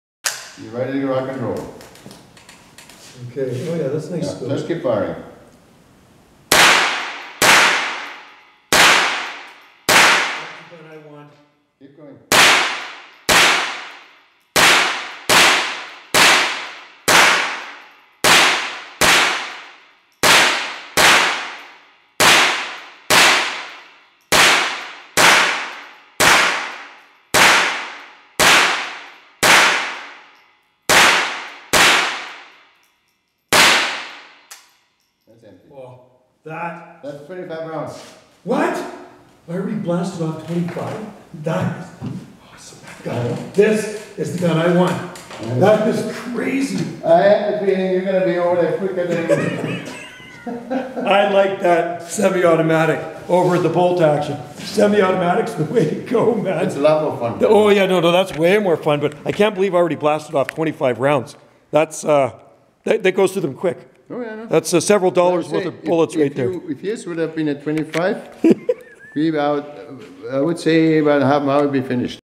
Testing a Ruger semi-automatic rifle at the Firing range
Ruger 10/22 semi-automatic rifle